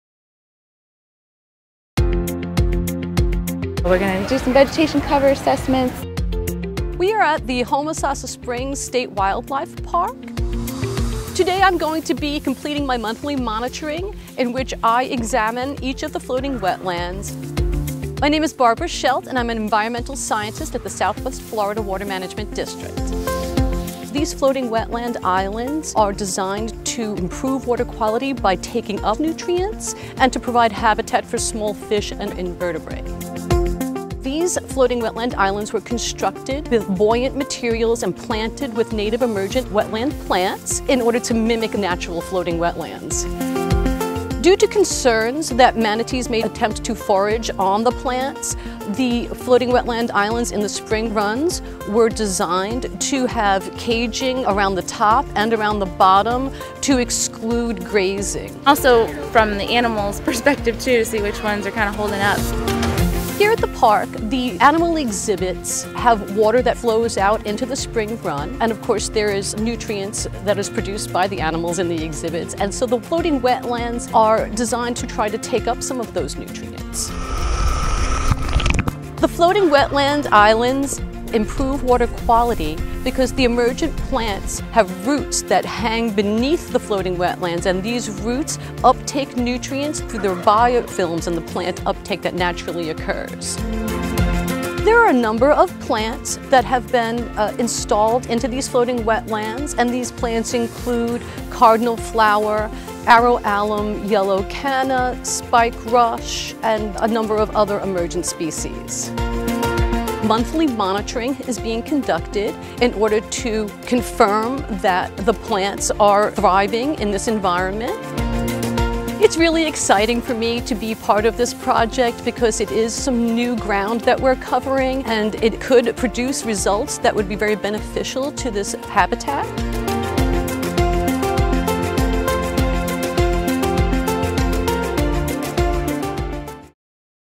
Floating Wetlands
Learn how some floating wetlands could help improve water quality.